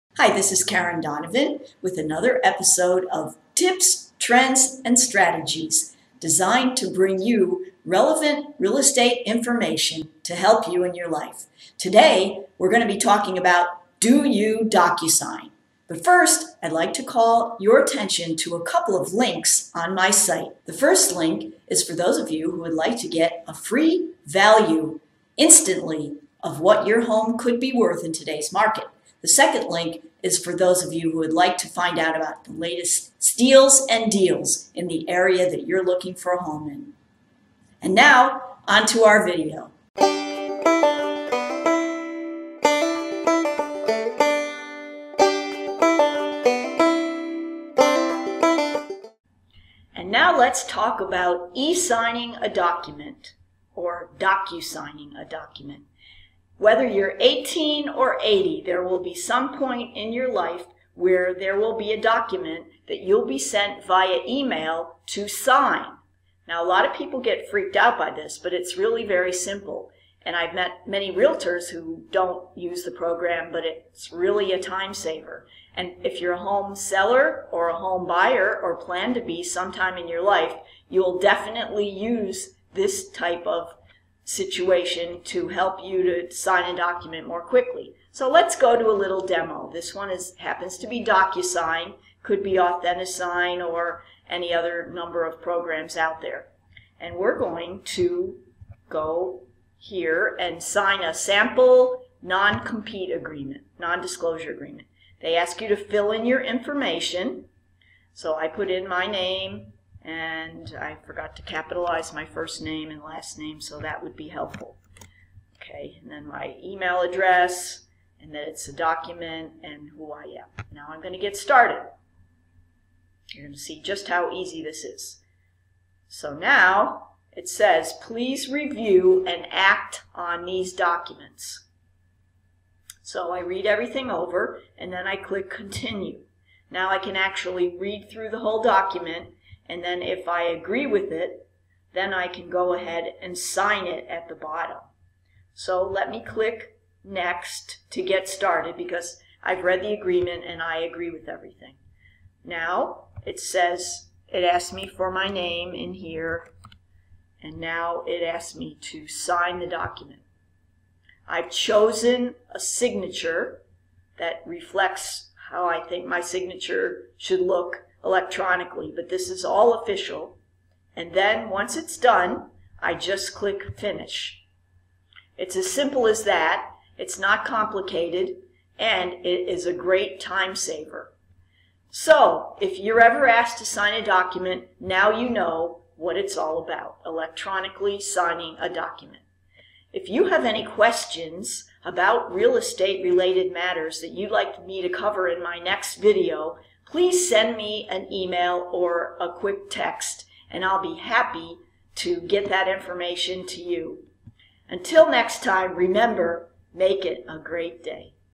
Long Island Real Estate: The scoop about DocuSign
Did you know you can sign a document electronically? With programs like DocuSign, you can e-sign a contract or other important documents by email. It’s incredibly simple once you learn how to do it. Plus, it saves a ton of time. Watch this video for a quick demo.

Buying a Long Island Home? Search ALL Homes For Sale

Selling Your Long Island Home?

Donovan Brothers Realty Inc.
LongIslandHomesList.com